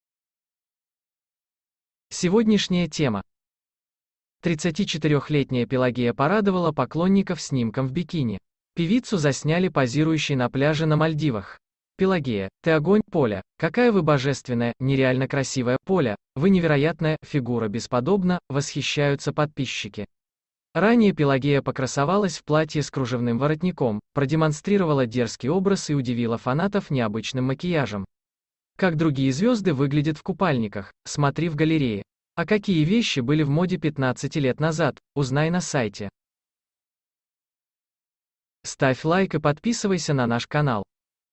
Пелагея покрасовалась в бикини на пляже
У певицы прекрасная фигура.